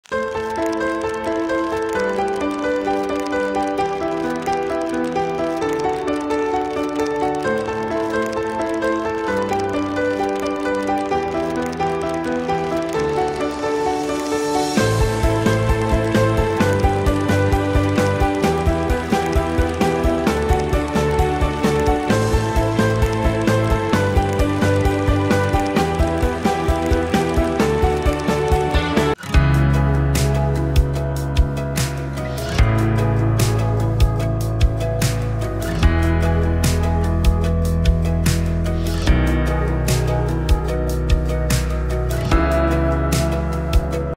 Evolution of Venom - FlipBook
Evolution of Venom in FlipBook Animation

Hello, I invite you to watch my video about flipbook animation, the video was made in Blender.
Share your impressions, wishes and ideas in the comments, I will be glad to everyone, have a nice day!!